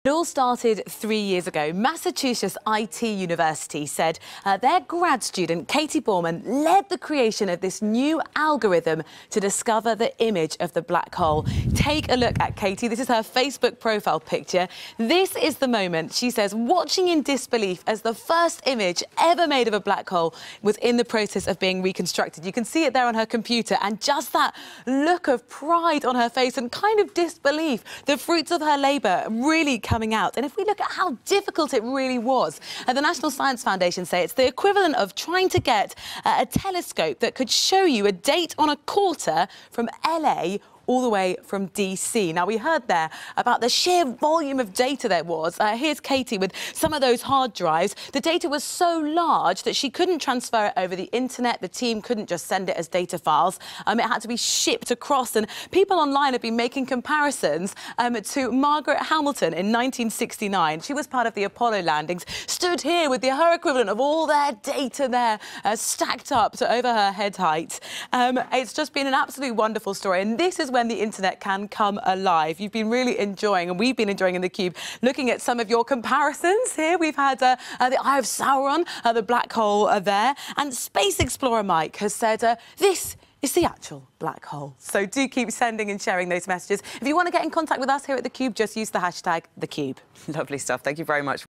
The 'Black Hole' photo - the story behind it
TheCube | The world has been celebrating the release of the first-ever image of a black hole - and one woman was instrumental in it all.

Rosie Wright and The Cube have the story behind the BlackHole photo.